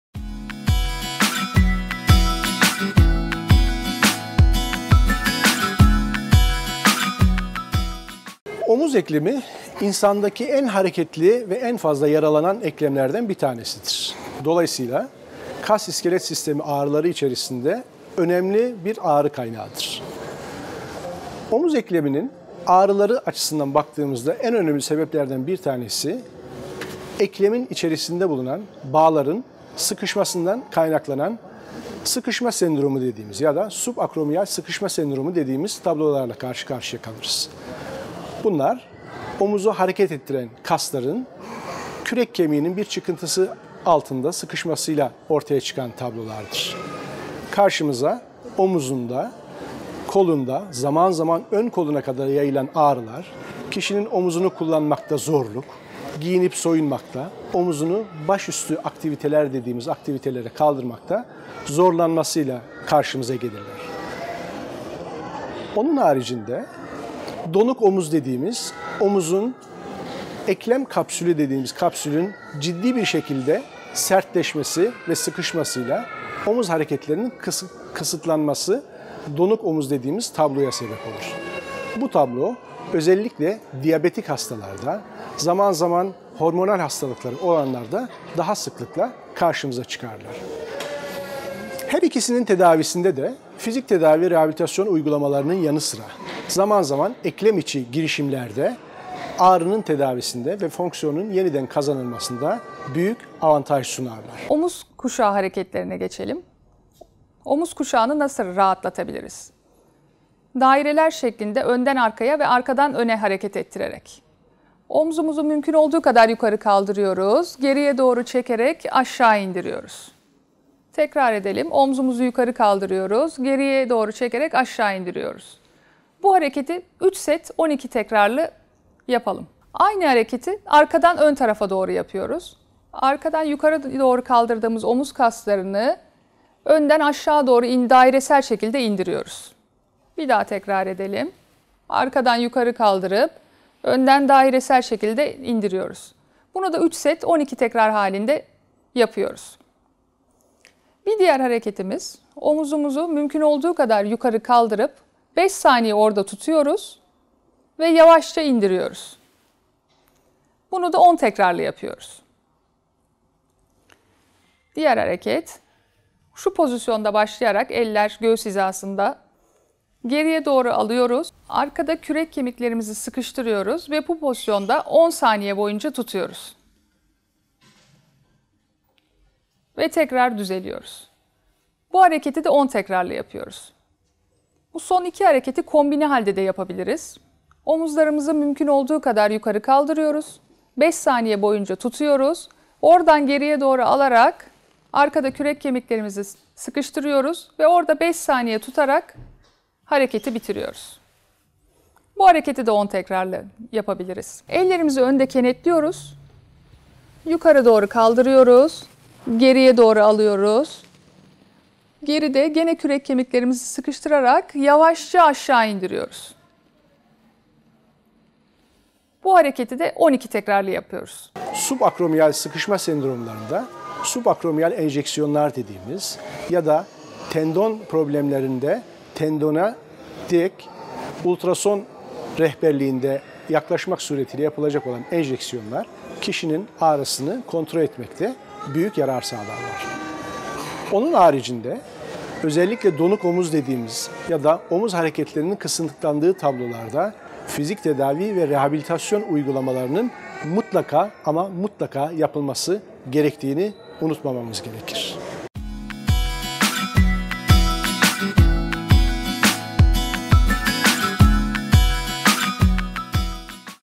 Omuz ağrısının nedenleri nelerdir? - Prof .Dr. Ümit Dinçer (Fiziksel Tıp ve Rehabilitasyon Uzmanı)
Memorial Bahçelievler Hastanesi Fizik Tedavi ve Rehabilitasyon Bölümü'nden Prof. Dr. Ümit Dinçer, omuz ağrısının nedenleri ve omuz ağrısını önleyen egzersizler hakkında bilgi verdi.